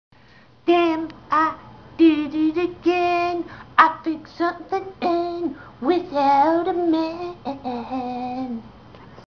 YAY!
Hopefully I fixed part of my ac issue without a man. one must enjoy the little things in life. Yes, I won't giveup my day job to become a singer. no worries!